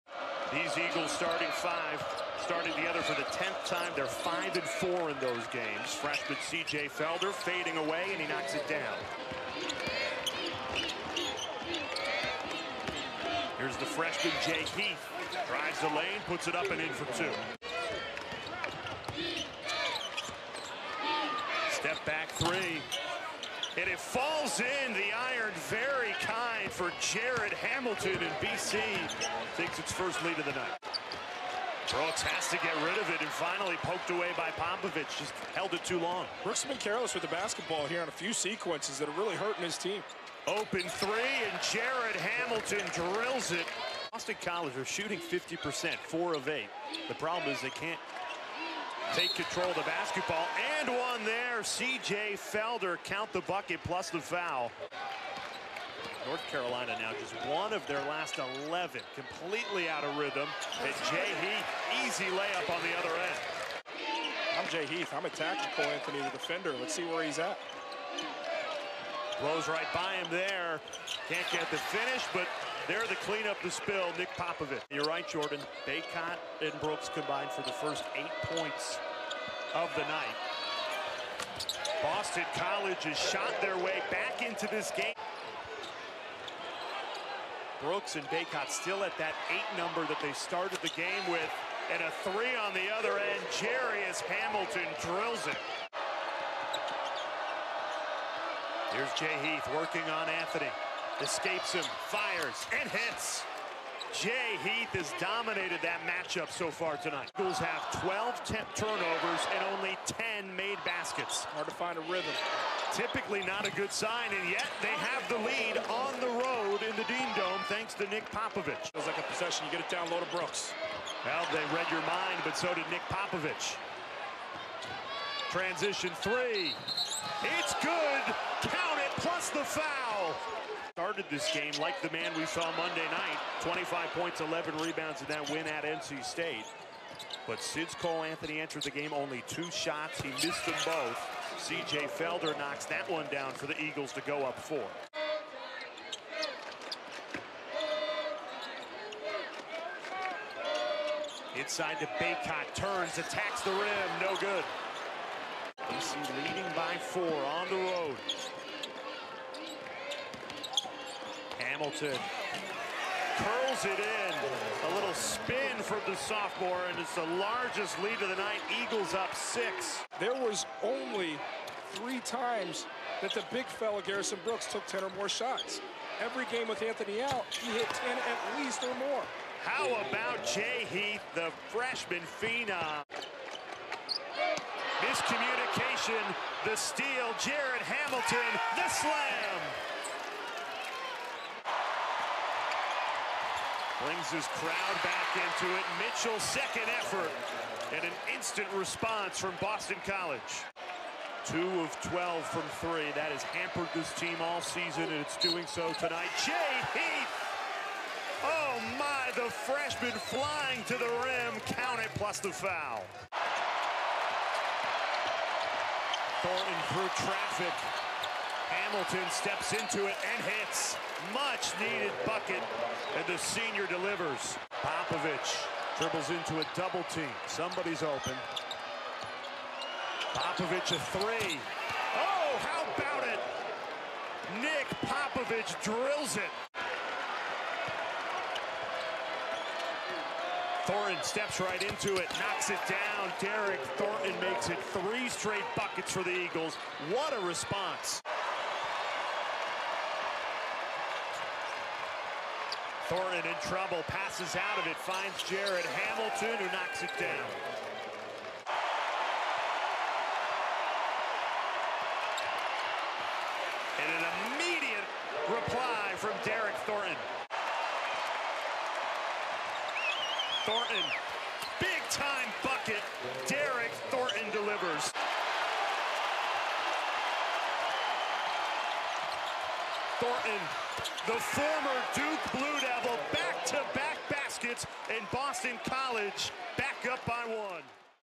Men's Basketball: UNC Highlights (Feb. 1, 2020)
Boston College defeated UNC 71-70 Saturday night in Chapel Hill.